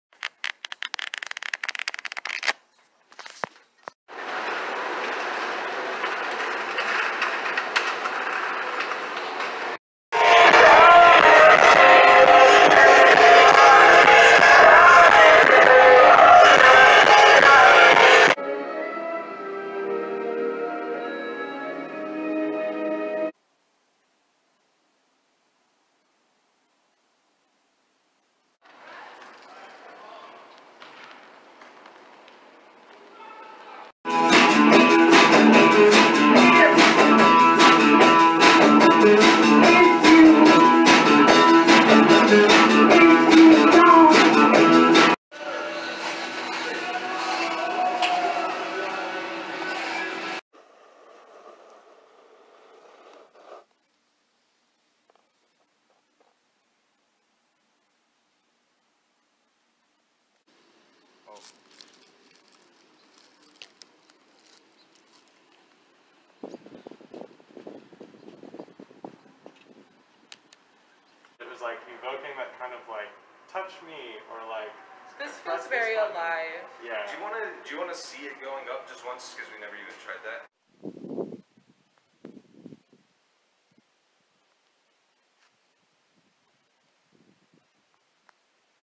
memory dump 21 to 22
1. tiny flip book
2. harold's
3. Deftones with Lanie
4. I worked at the van gogh exhibit and made money, but was sweaty
5. Fluorite mined in NM
6. Art show I helped curate and install. A band called Big Girls preformed in the gallery
7. burger by a talented and artistic and cool person (it was delicious)
8. paper mache mask I made for a workshop I facilitated (my job is art now)
9. playing with Gus's cool camera. W/ Andrea and Gus at the park collecting park sounds
10. I helped build an interactive permanent sculpture that tells stories when you push a button
11. my story of journey, how did I get here?